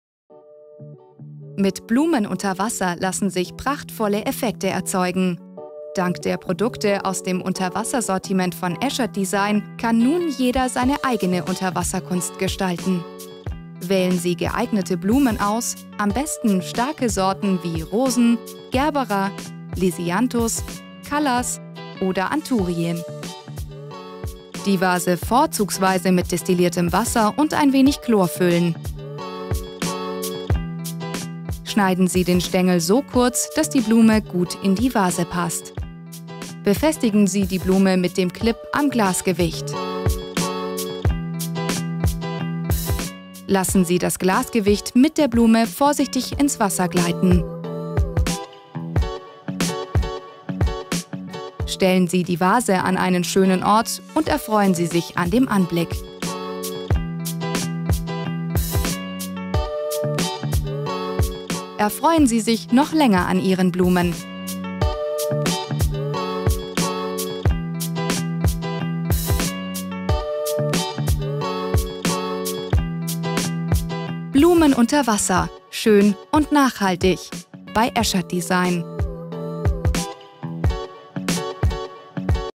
Blumen unter Wasser - Schön und nachhaltig
Mit Blumen unter Wasser lassen sich prachtvolle Effekte erzeugen. Dank der Produkte aus dem Unterwassersortiment von Esschert Design kann nun jeder seine eigene Unterwasserkunst gestalten. Erfreuen Sie sich noch länger an Ihren Blumen!